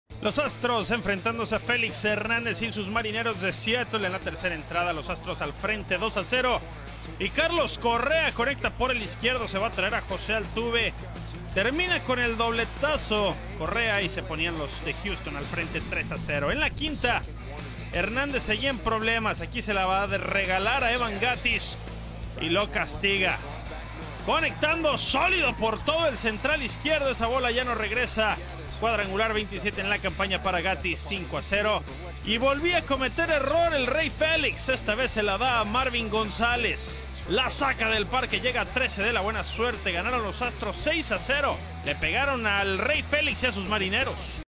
Astros maltrata al 'Rey Félix' y Marineros
Los Astros le dieron duro al 'Rey' Félix Hernández, al grado de 'explotarlo' de la loma en la quinta entrada, para llevarse una valiosa victoria ante un rival directo en la pelea por un boleto de comodín de la Americana.